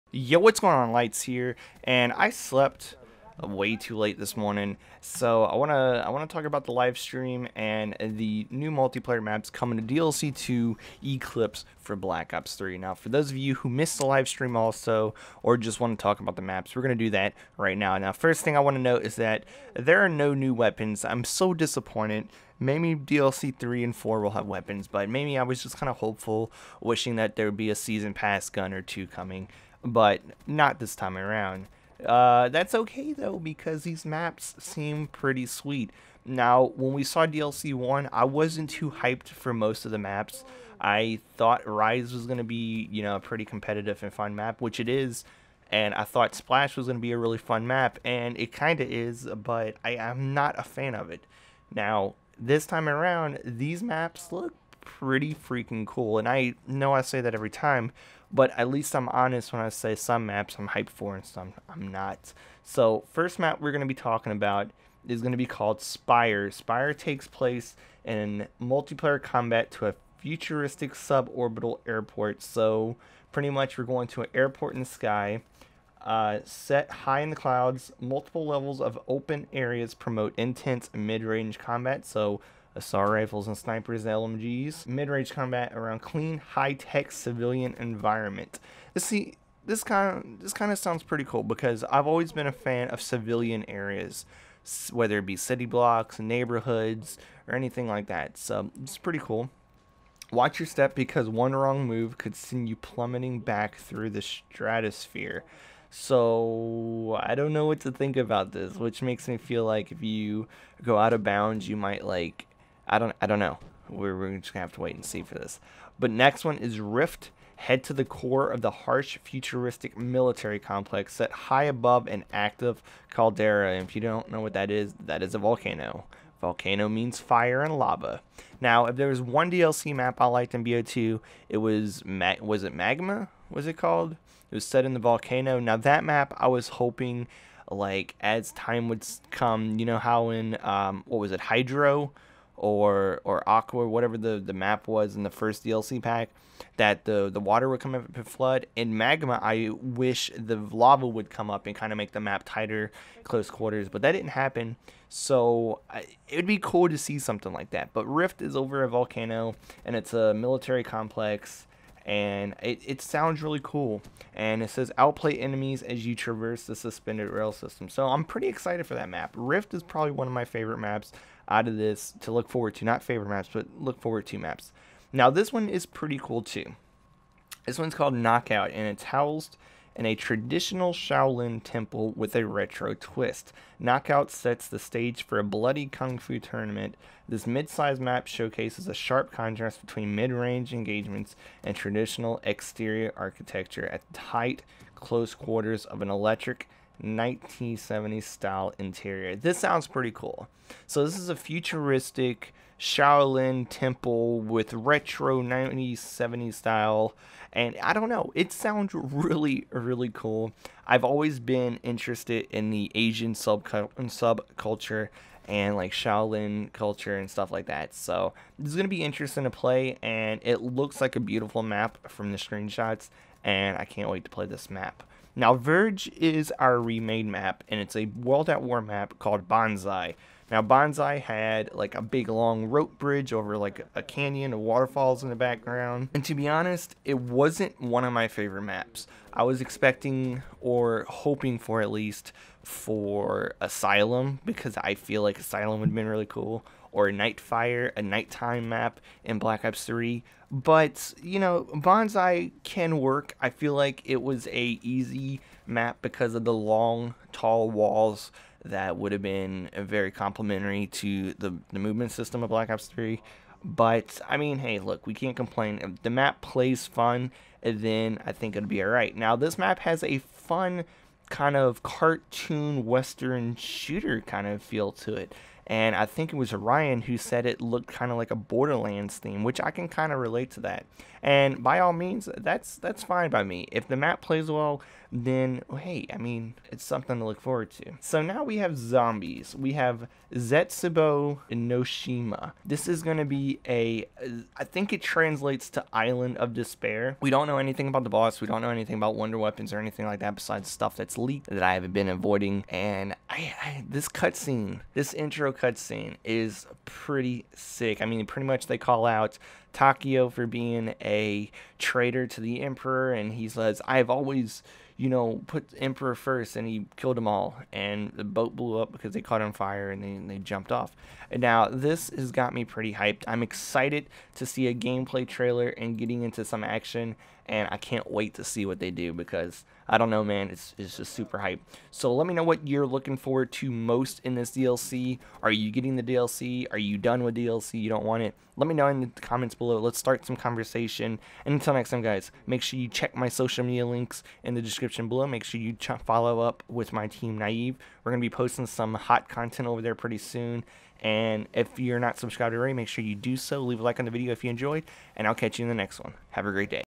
My Thoughts on Black Ops 3: DLC 2 "Eclipse"
So much hype around BO3's DLC 2: Eclipse. I talk about the 4 multiplayer maps: Rift, Verge, Spire, and Knockout as well as the zombies map: Zetsubou No Shima. I am so hyped to get into these maps, and explore the new zombies experience. What are you most hyped for in the upcoming DLC?